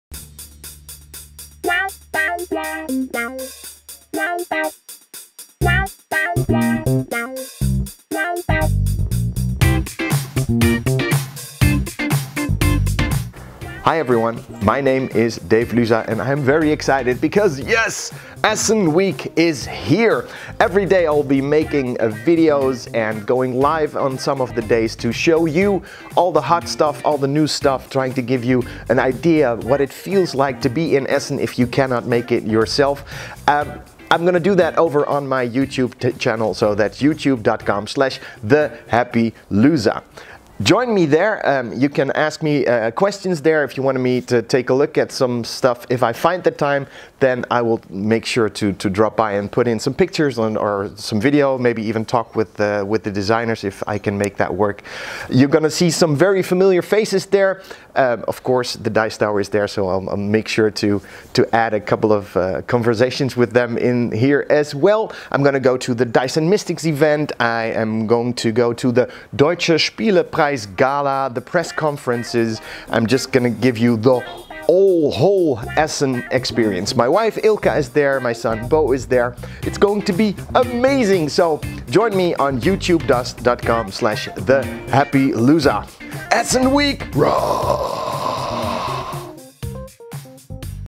Essen Vlog promo
Can´t make it to Essen Spiel this year? Don´t worry, i have got you covered.
Starting tonight (monday october 22nd) I will start putting out daily videos and live streams. Join me tonight for the first one.

Let me know if there is something you want me to take a look at, and I will try to fit it in. This week is epic already!

ESSEN WEEK IS HERE!